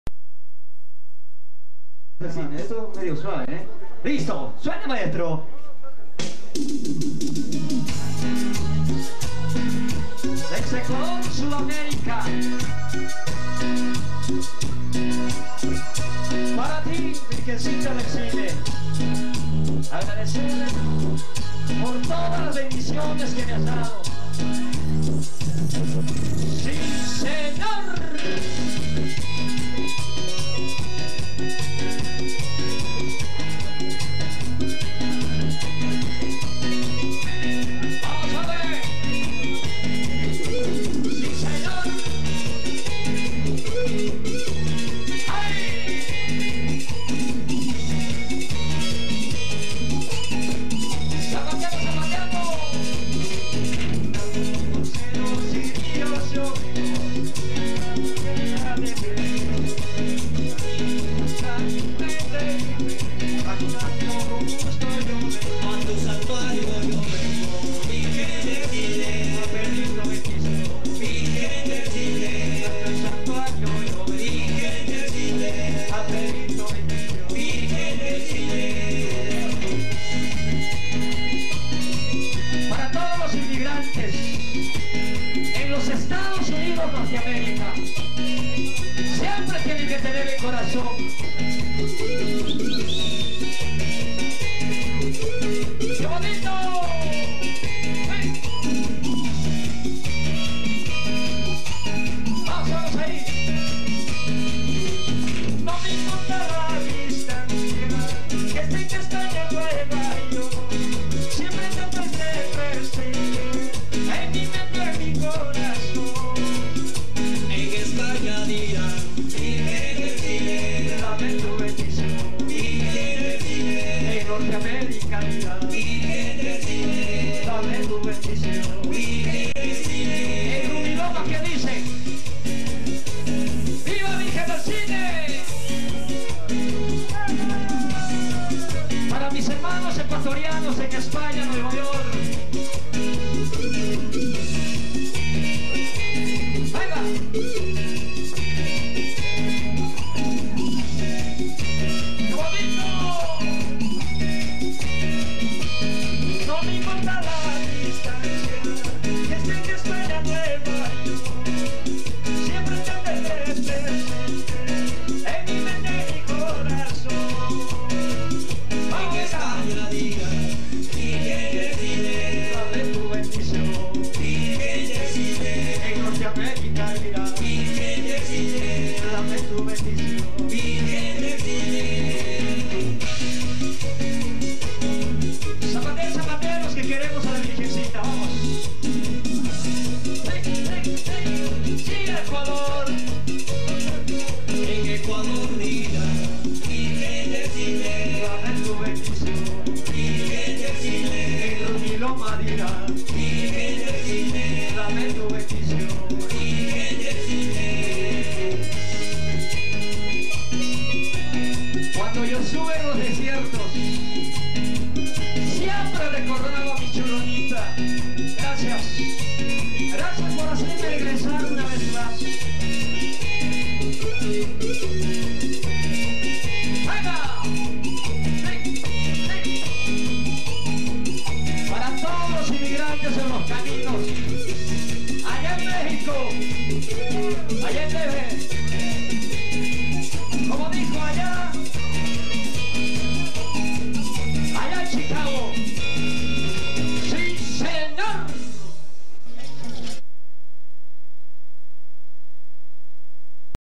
Manuelito El Cantor Cañari
manuelito en las fiestas de rumiloma huayrapungo en honor ala virgen del cisne 2011